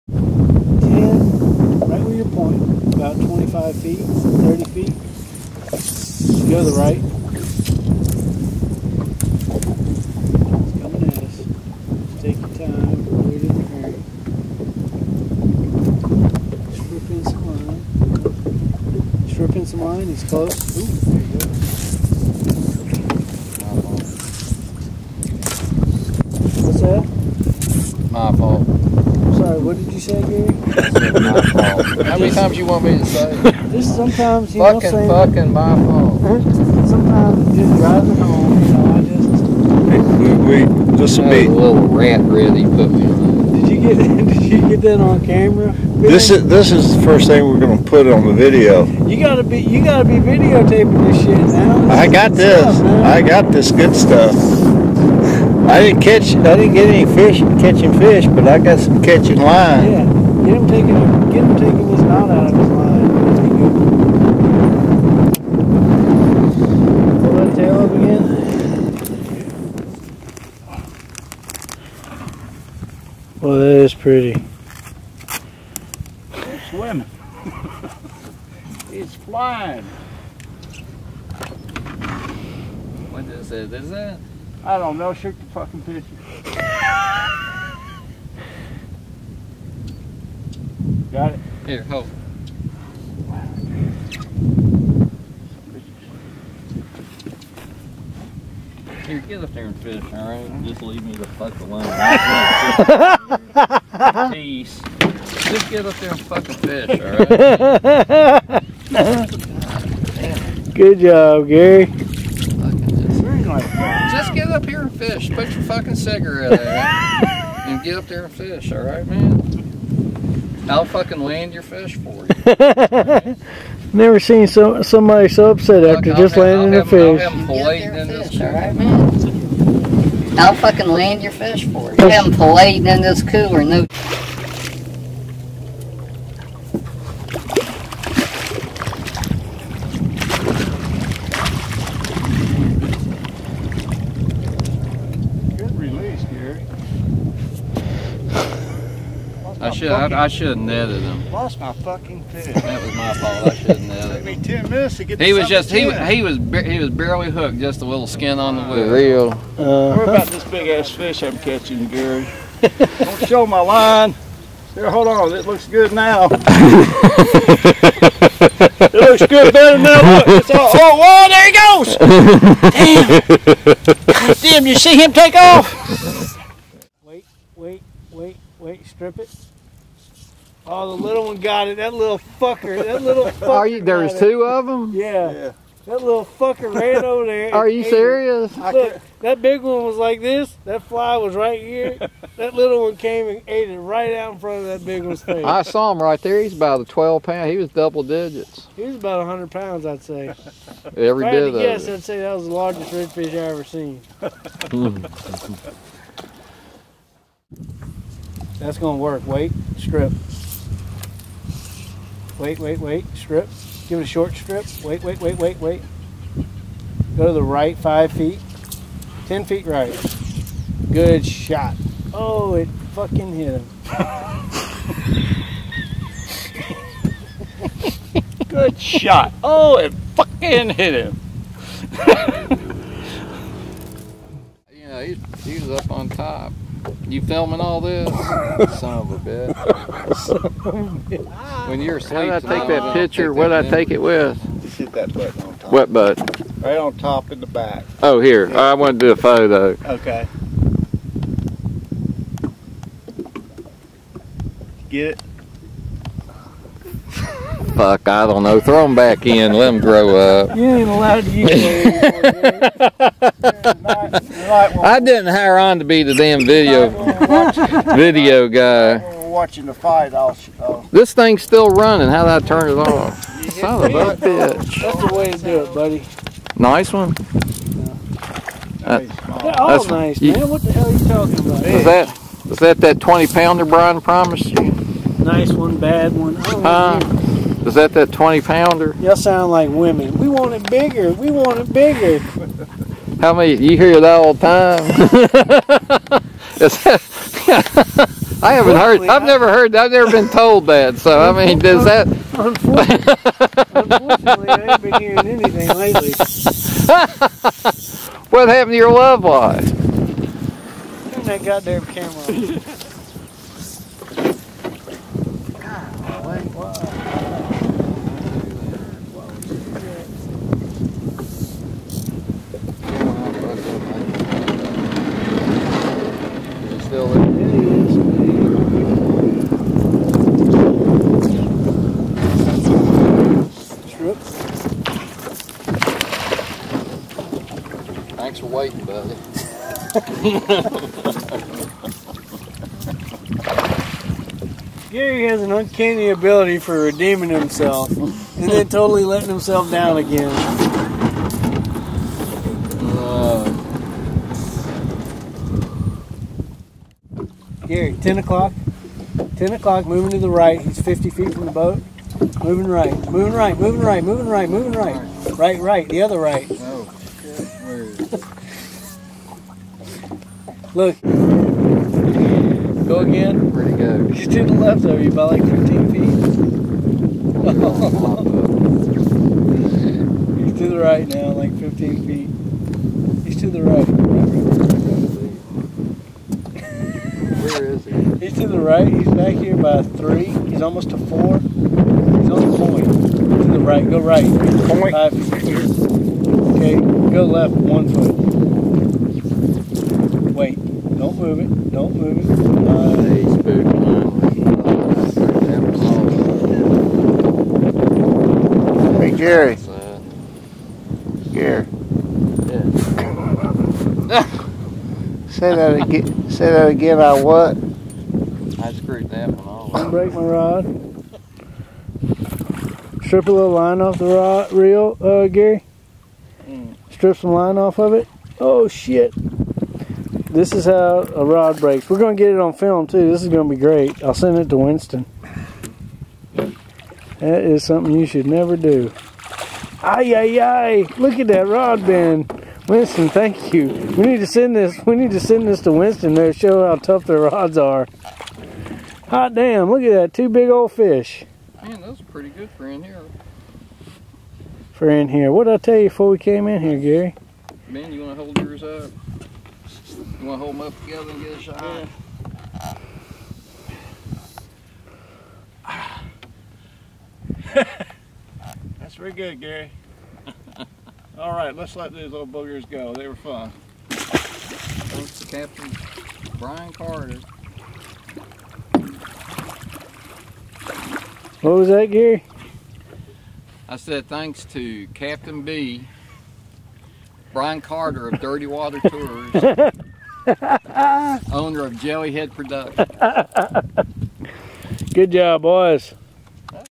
Red Fish with Capt B.
Out takes from Red fishing trip to New Orleans on Capt. B's Dirty Water Guide Service.